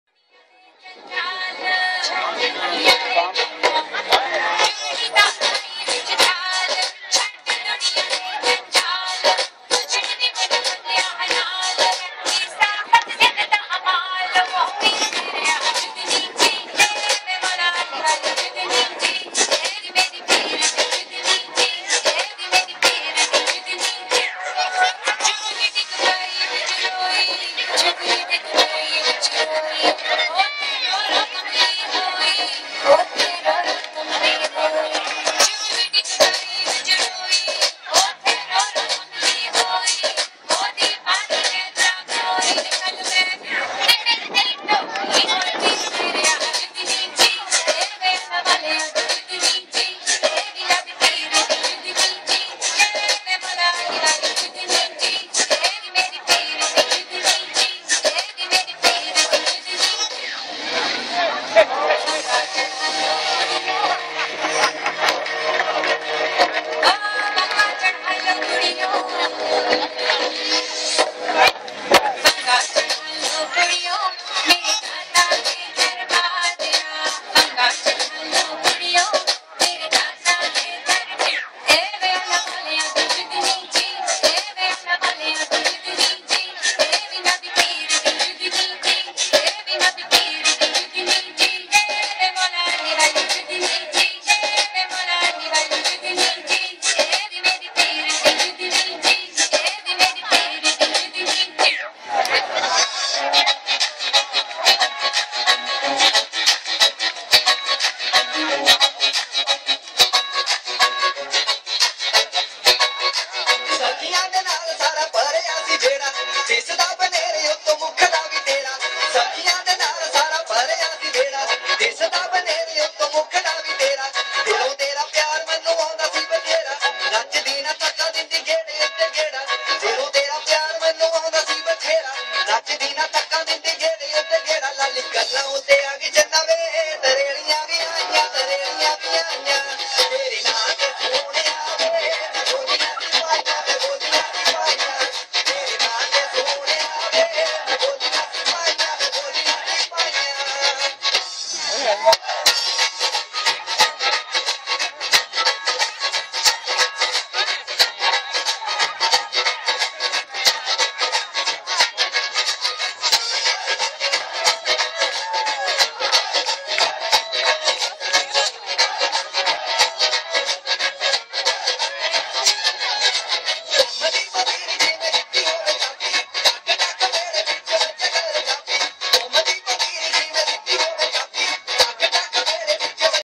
Indian Dancing
Charlotte and Mama dancing with the Indian Troop at the El Cerrito July 4th Party.